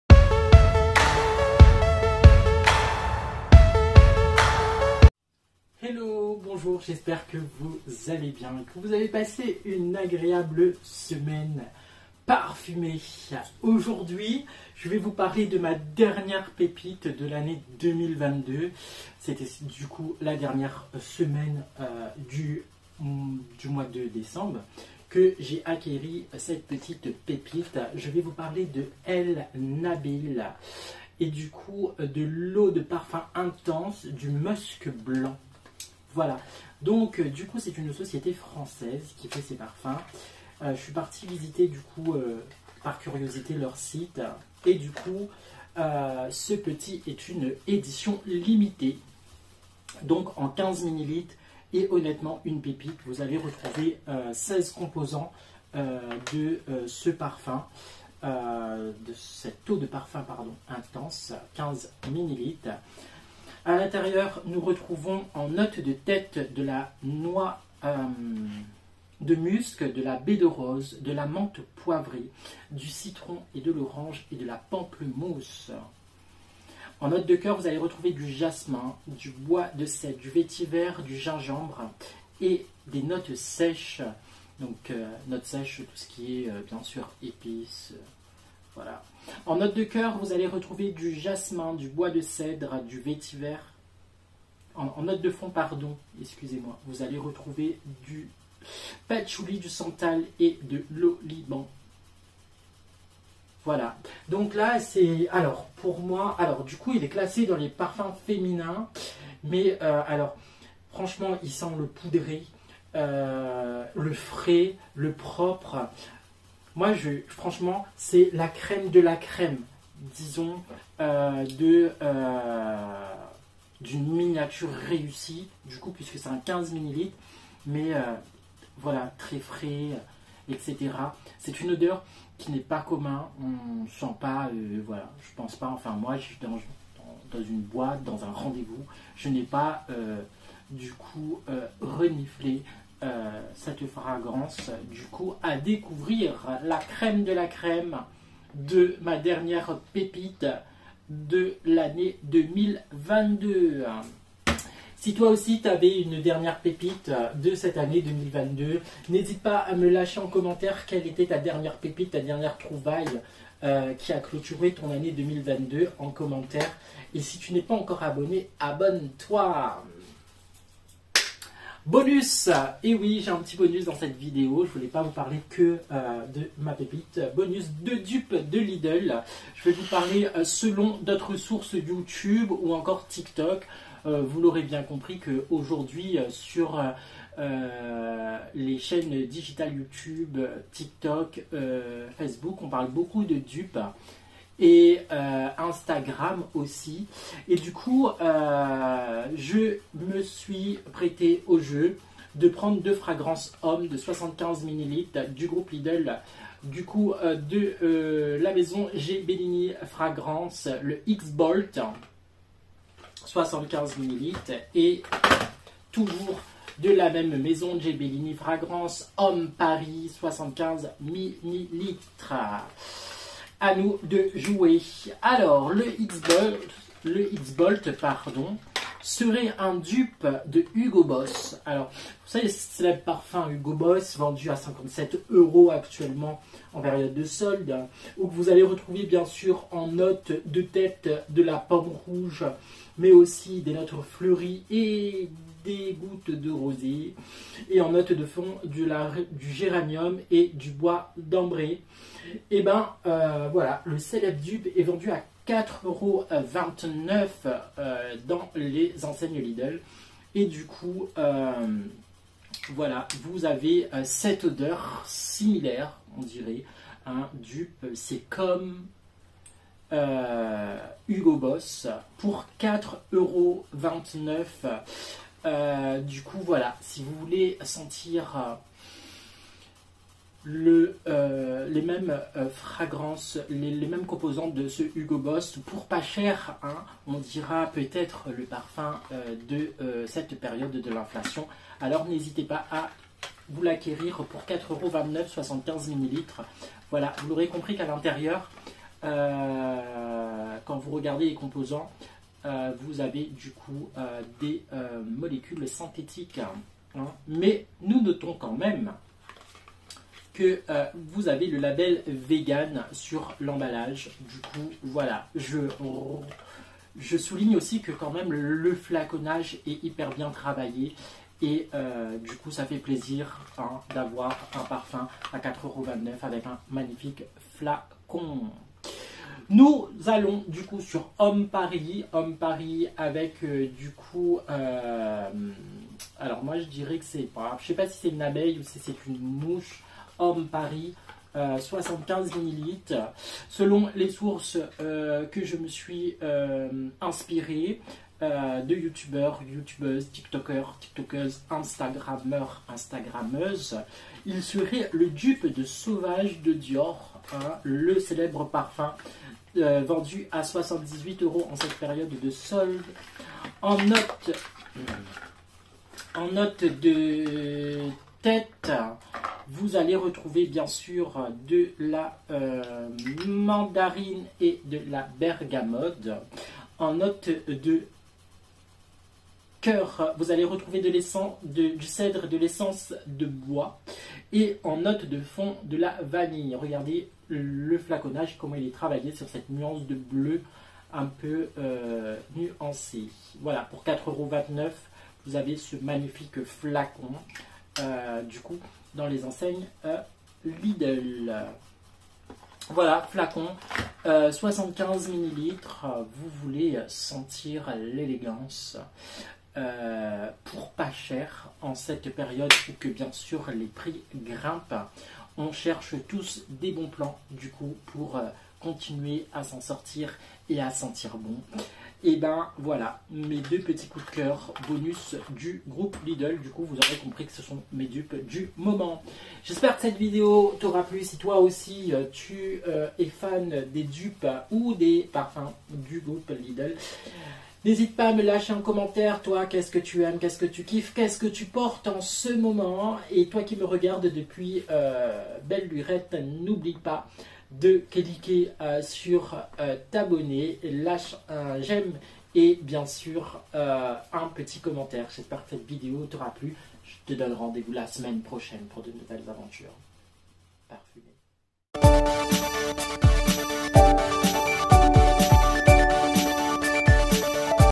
Ma dernière acquisition de 2022 ! bonus parfum lidl.
bonjour.

petite vidéo de ma dernière acquisition de 2022 .

je vous présente également mes 2 coup cœur de parfums lidl .

bonne vidéo.